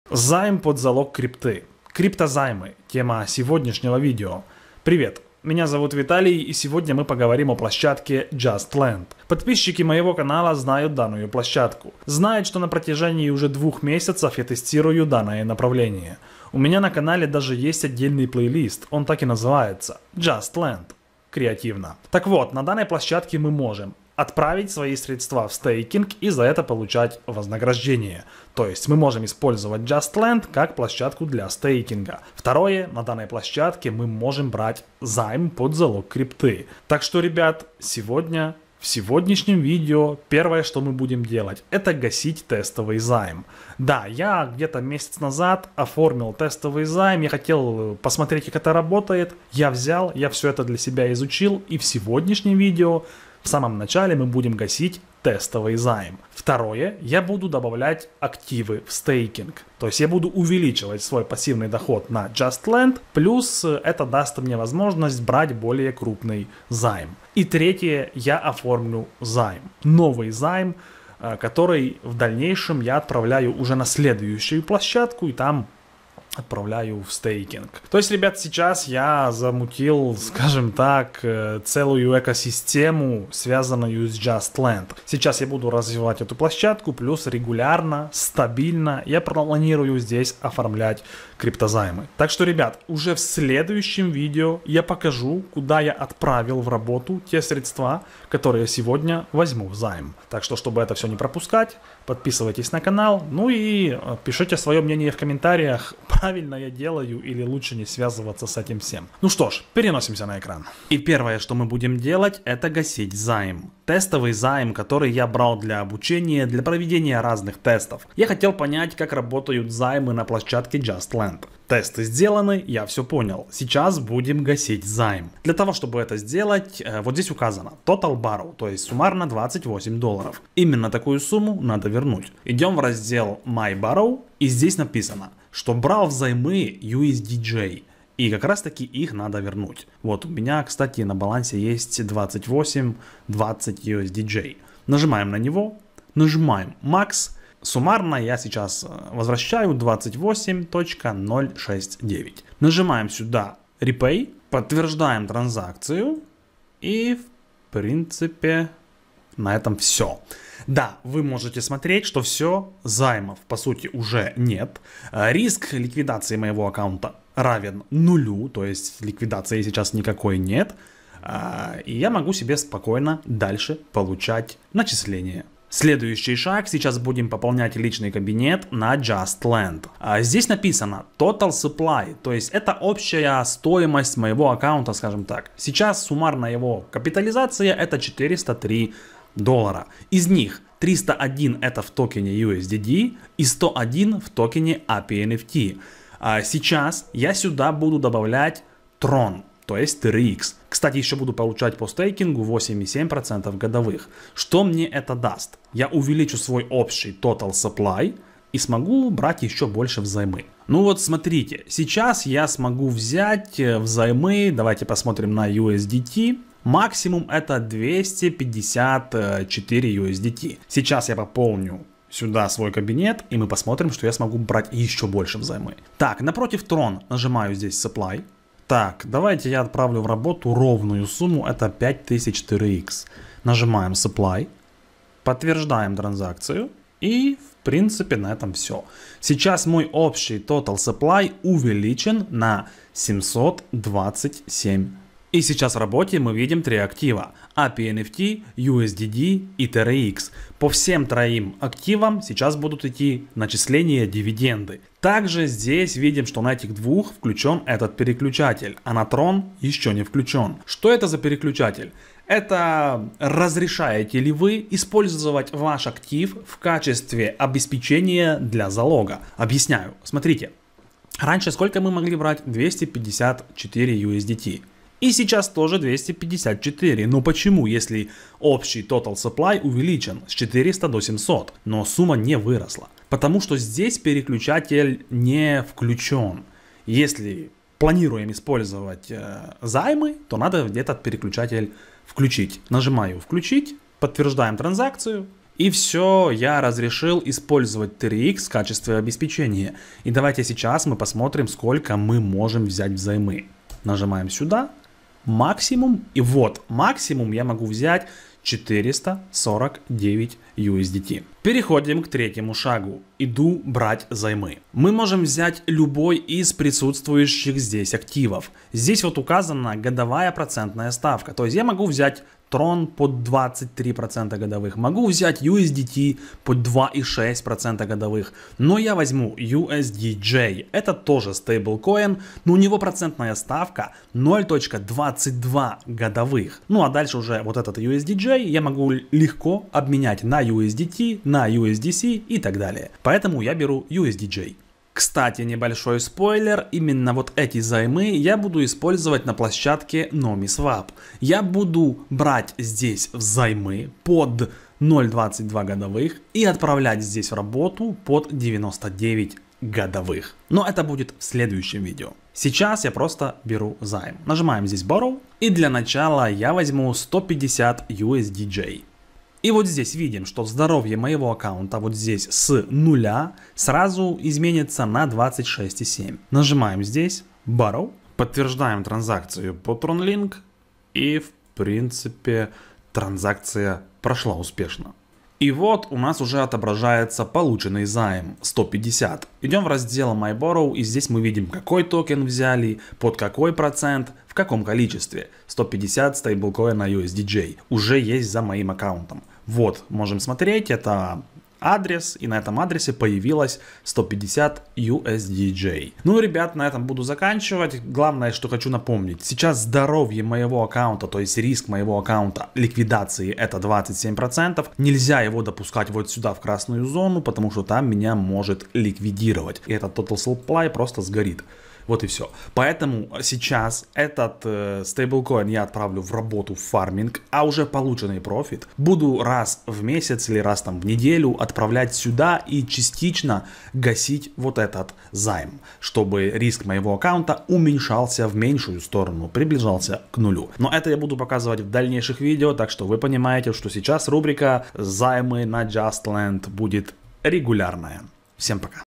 КРИПТОЗАЙМ НА JUSTLEND. TRX, USDJ
JustLend ЗАЙМЫ.
Месяц уже тестирую функцию займов.
В этом видео поговорим о займах на JustLend.

В ЭТОМ ВИДЕО:
- Погашение тестового займа.
- Отправка TRX в стейкинг
- Оформление нового займа.

Если у Вас остались вопросы, задавайте их в комментарии под видео, или ниже Вы можете найти мои контактные данные, и связаться со мной.